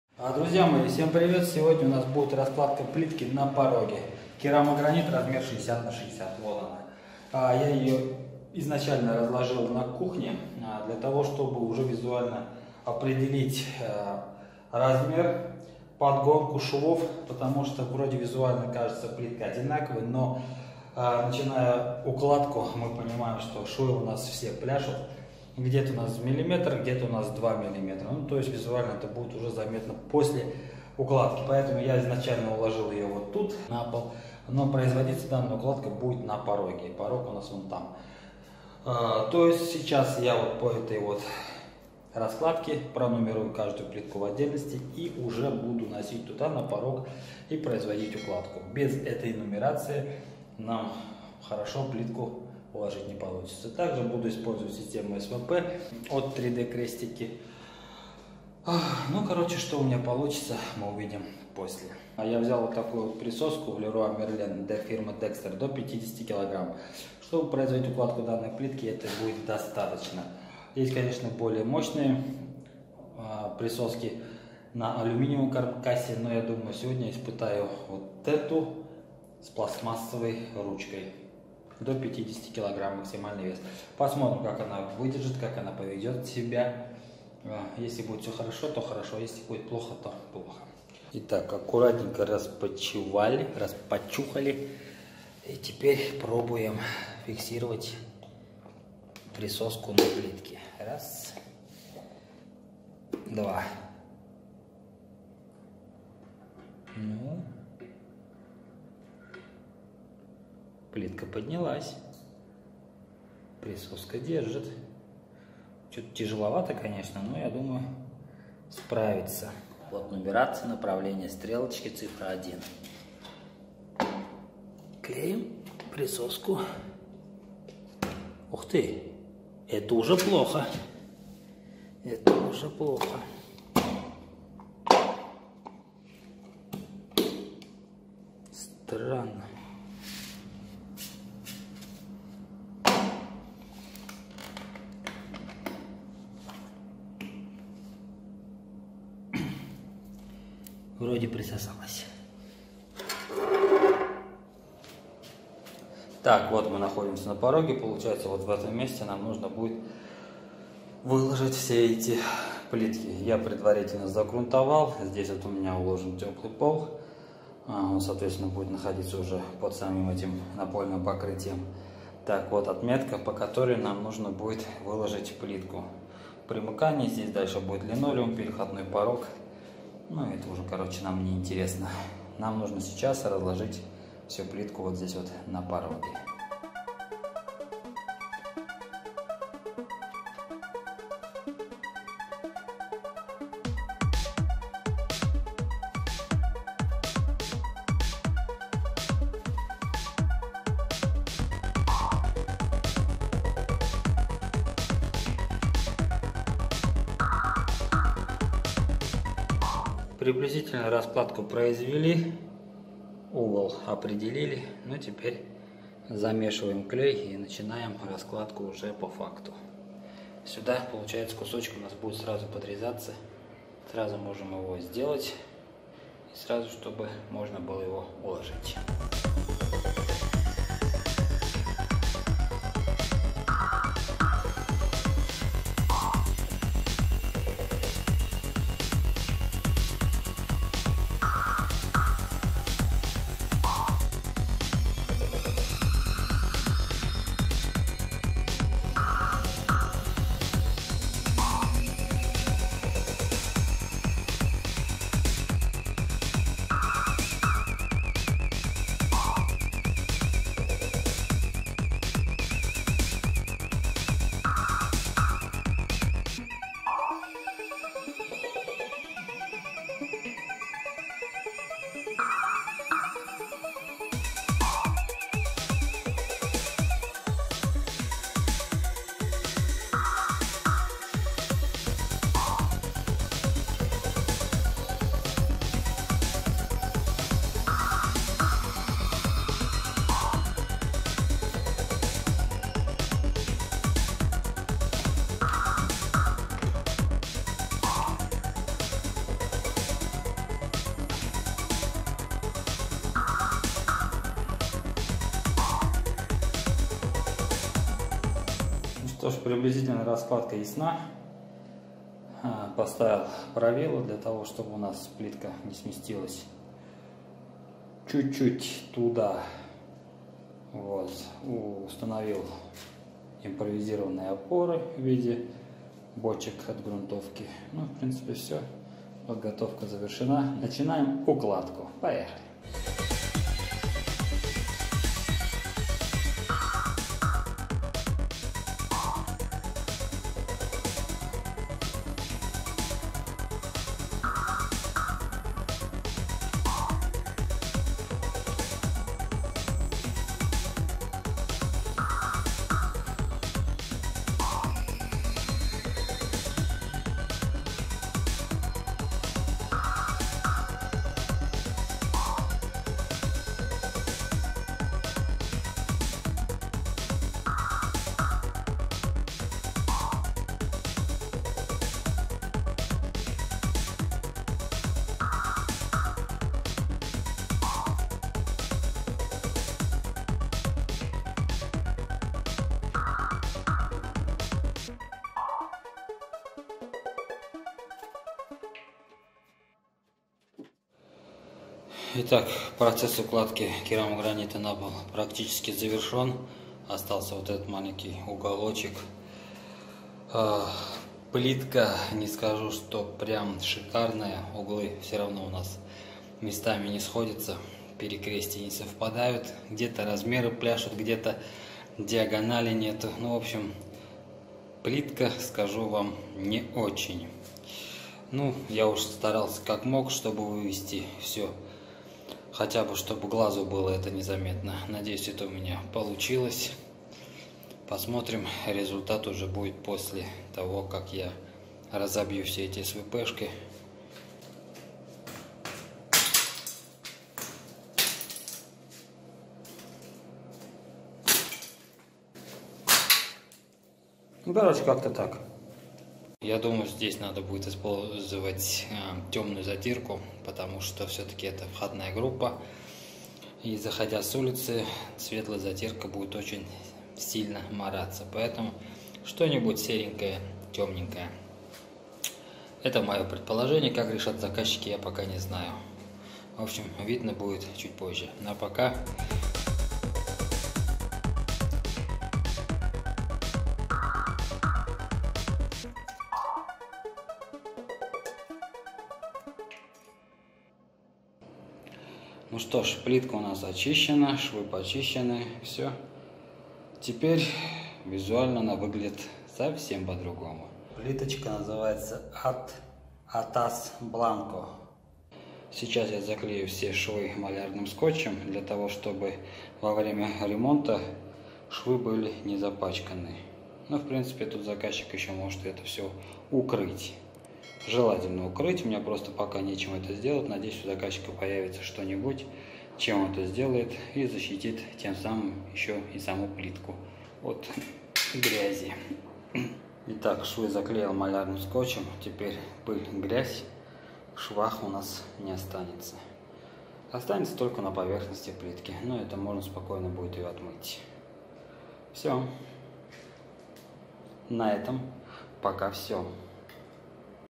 Attas Blanco правильная укладка плитки на порог.
Attas Blanco 60.4x60.4 керамическая плитка напольная из керамогранита, полированная, белая, Испания.

Я всегда рекомендую на входной группе использовать керамическую плитку или керамогранит.
Потому-что:
1. Проще мыть
2. Не боится  воды
3. При использовании теплого пола (тоже рекомендую) можно сушить обувь.
4. Огромный выбор цвета и форм.
5. И наконец-то просто красиво!

Во время укладки использовалась керамическая плитка напольная из керамогранита Attas Blanco 60.4x60.4.
Применялась система выравнивания плитки от компании 3dkrestiki.
Для удобного перемещения плитки и укладки использовалась присоска Dexter из магазина Леруа Мерлен.

А так же много забавных моментов во время работы.
Советы, рекомендации.
и Т.Д.

Жду от Вас обратную связь)